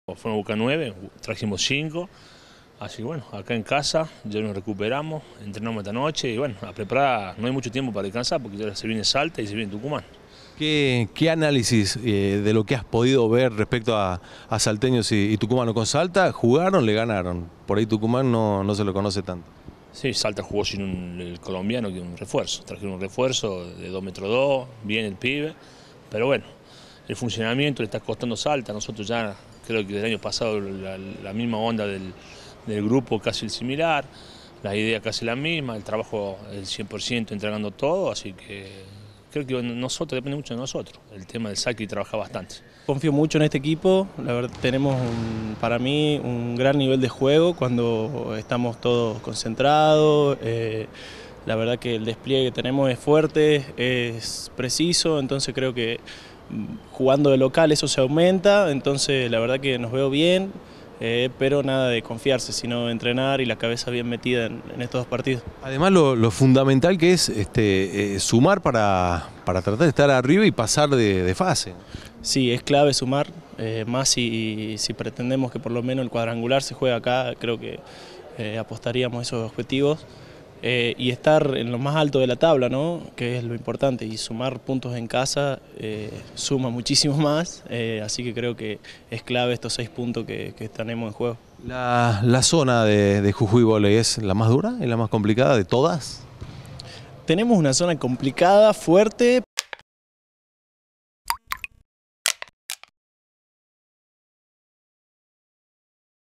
Jujuy Vóley debuta este jueves de local por la liga A2
A partir de las 22.00, Fundación recibirá a Salta voley por el 3º wekeend.